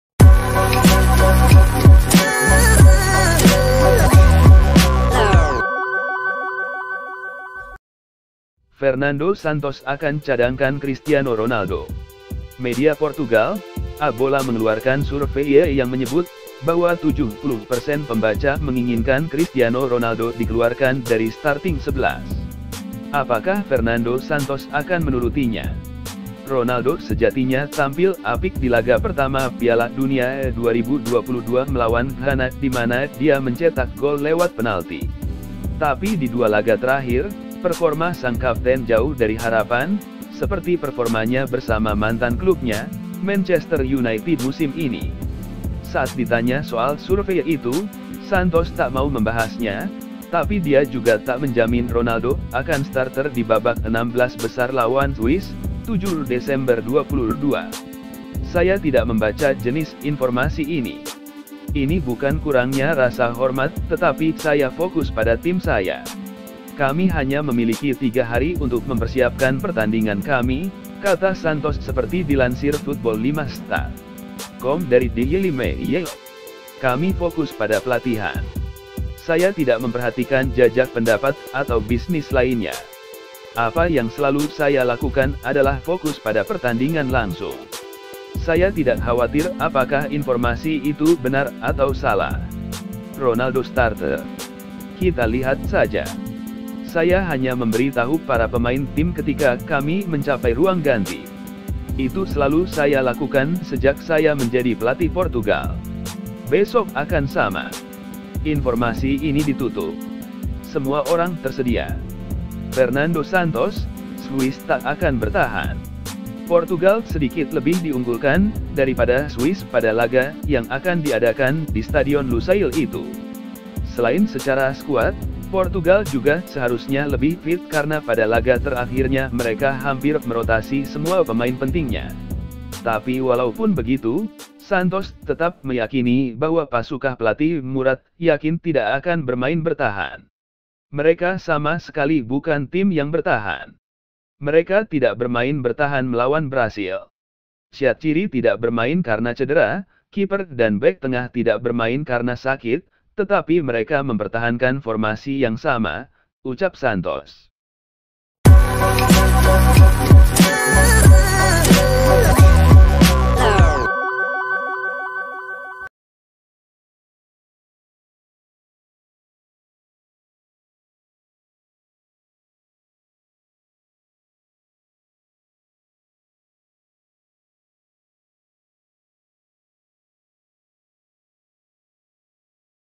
CRISTIANO RONALDO AKAN DICADANGKAN OLEH FERNANDO SANTOS?
Fernando Santos akan Cadangkan Cristiano Ronaldo?

Media Portugal, A Bola mengeluarkan survei yang menyebut bahwa 70 persen pembaca menginginkan Cristiano Ronaldo dikeluarkan dari starting XI. Apakah Fernando Santos akan menurutinya?

Ronaldo sejatinya tampil apik di laga pertama Piala Dunia 2022 melawan Ghana dimana dia mencetak gol lewat penalti. Tapi di dua laga terakhir, performa sang kapten jauh dari harapan, seperti performanya bersama mantan klubnya, Manchester United musim ini.

Saat ditanya soal survei itu, Santos tak mau membahasnya, tapi dia juga tak menjamin Ronaldo akan starter di babak 16 besar lawan Swiss (7/12/22).